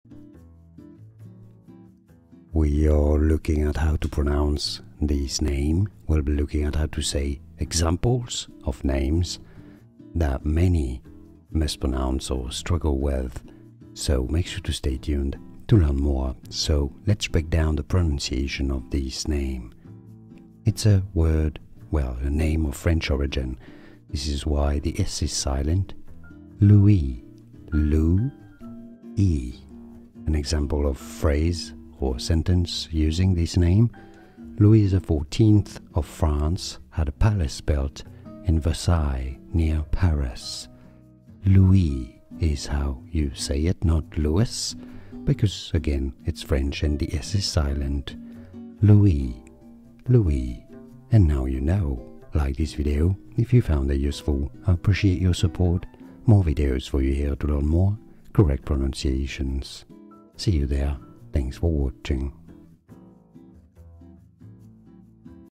How to Pronounce Louis (In English and French)
This video shows you How to Pronounce Louis (In English and French), pronunciation guide.

00:00 - Pronunciation Intro
00:16 - How to Pronounce Louis (In English and French)
00:46 - More Difficult Pronunciations

Louis is the French form of the Old Frankish given name Chlodowig and one of two English forms the other being Lewis (/ˈluːɪs/).
Etymology
The name Louis (through the intermediate form Clovis) derives from the Frankish name ᚺᛚᛟᛞᛟᚹᛁᚷ (in runic alphabet) or *Hlōdowik or *Hlōdowig (in Latin alphabet). Traditionally, this name is considered to be composed of two elements, deriving from both Proto-Germanic *hlūdaz ("loud, famous") and *wiganą ("to battle, to fight") respectively, resulting in the traditional practice of translating Clovis' name as meaning "famous warrior" or "famous in battle".